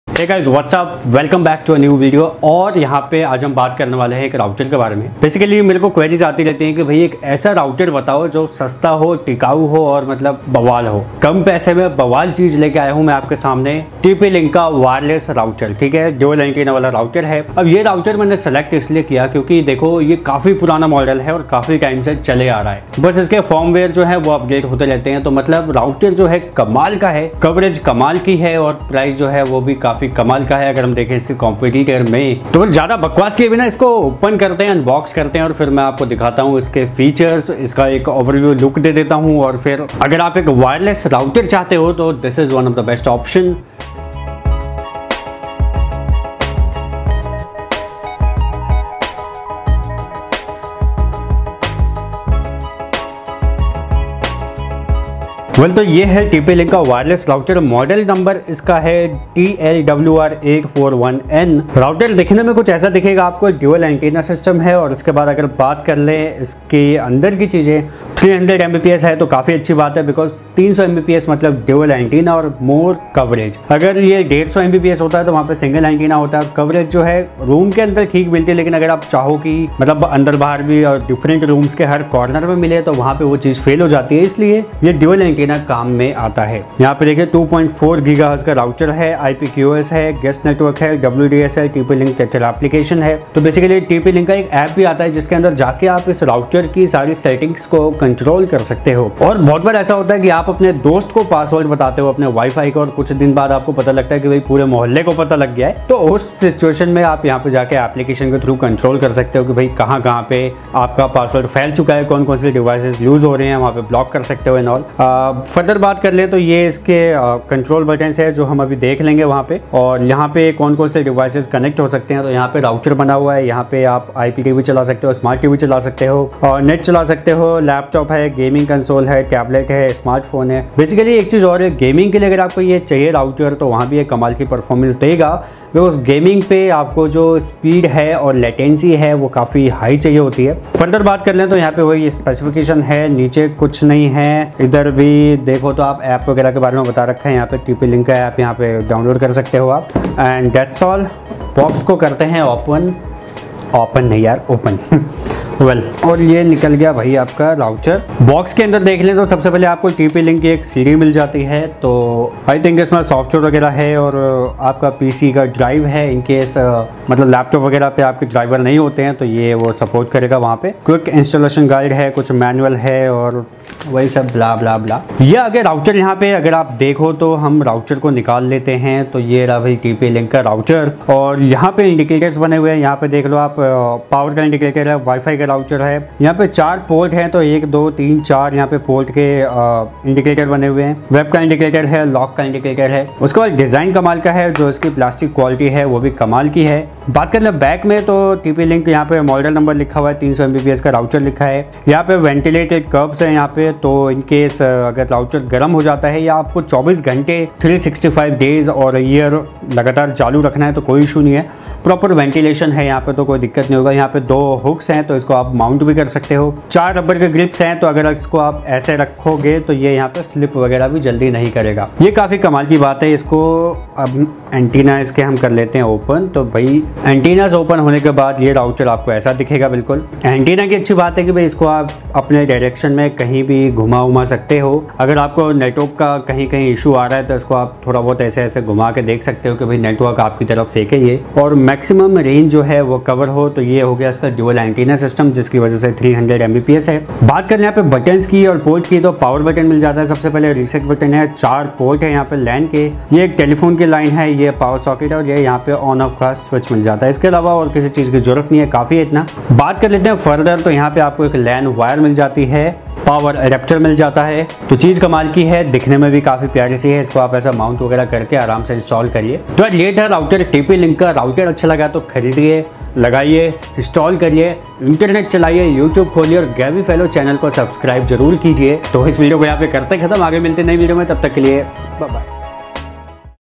Best Wireless Router | TP LINK Best & Affordable Dual Antenna 300 Mbps Router
🙏 Namashkar Fellows 🙏

Iss video mai aapse baat karne wala hoon TP Link 300 Mbps TL-WR841N Router ke baare main !!

Camera : Canon 80D
Mic : Snowball White/Clip Mic
Editor : Final Cut Pro X
Laptop : MacBook Pro
Sound Editor : Audacity

Namaskar Fellows 🙏